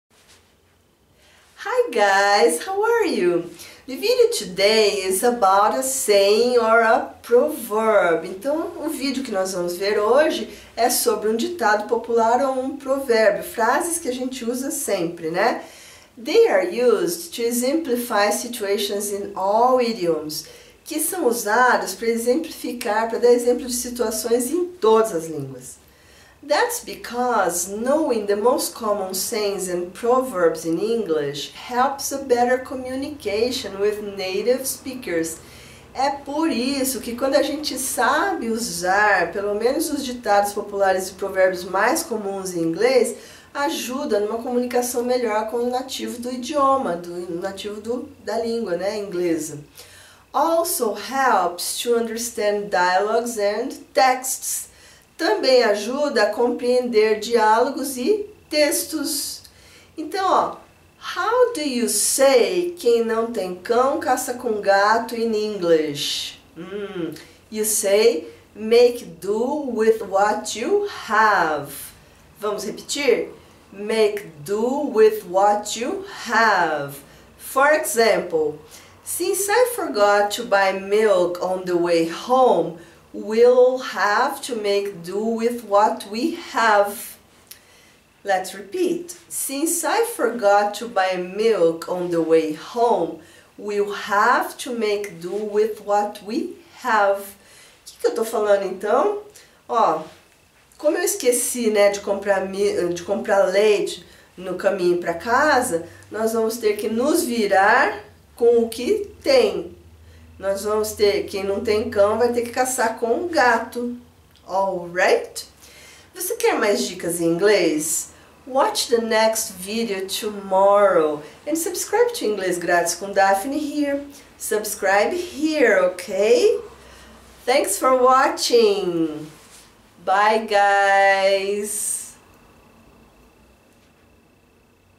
como se diz QUEM NÃO TEM CÃO CAÇA COM GATO em inglês - inglesgratiscomdaphny
Neste vídeo vamos aprender como se diz QUEM NÃO TEM CÃO CAÇA COM GATO em inglês e também vamos ver um exemplo bem ilustrado que ajuda na fixação dessa expressão.